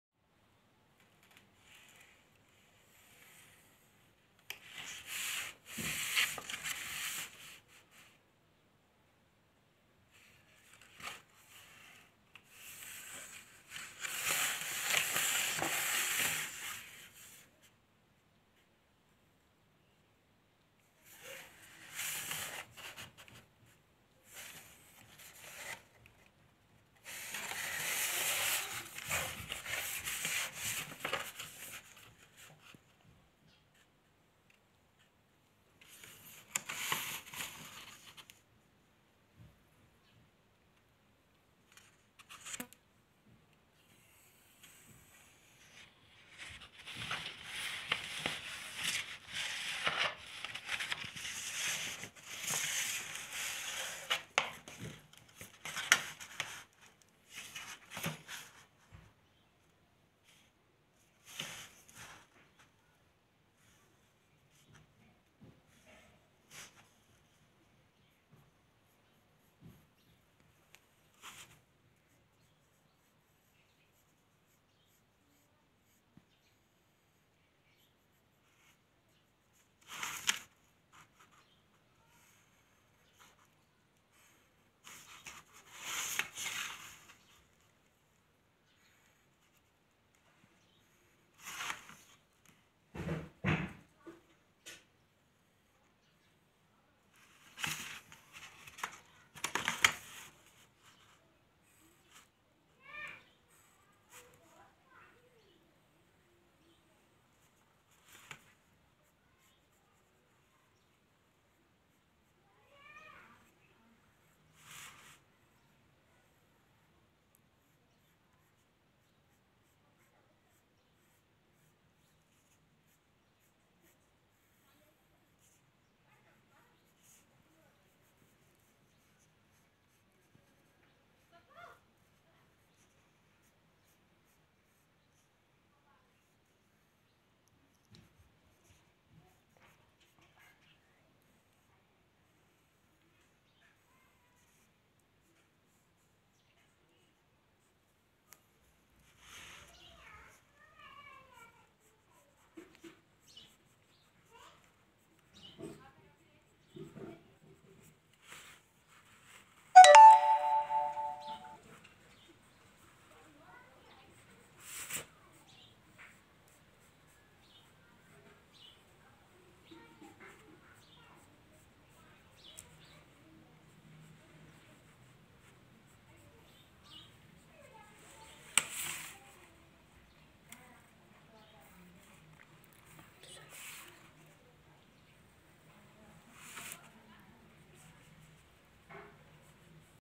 SLOGAN WRITING
SLOGAN WRITING REGARDING CELEBRATION OF 400TH BIRTHDAY OF SHRI GURU TEG BAHADUR JI